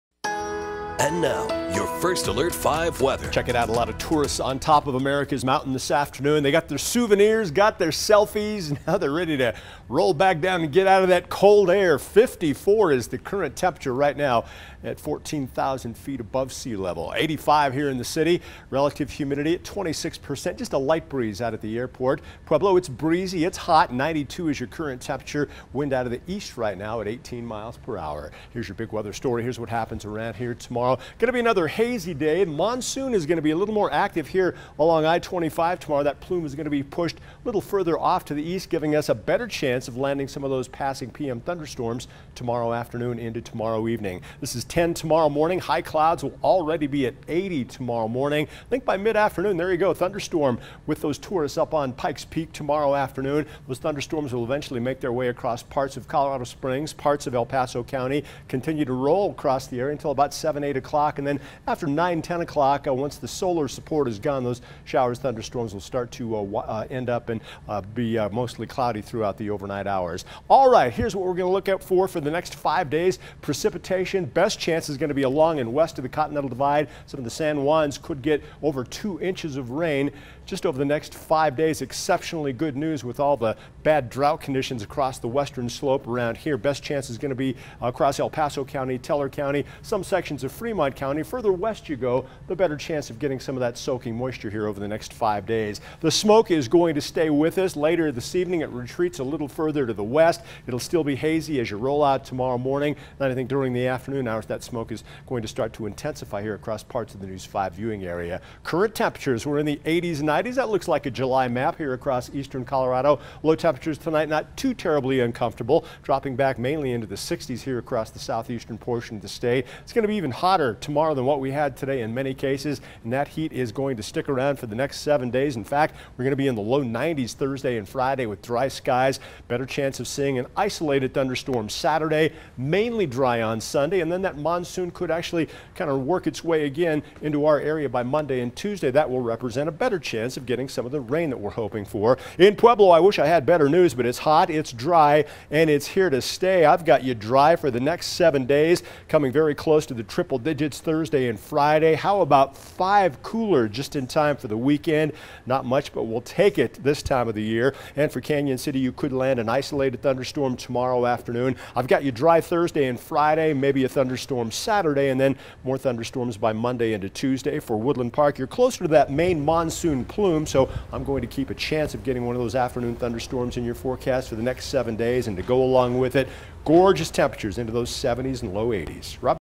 Better chances for rain Wednesday, but not for everyone
With a better push of monsoon moisture on Wednesday, hazy morning sunshine will give way to scattered showers and thunderstorms.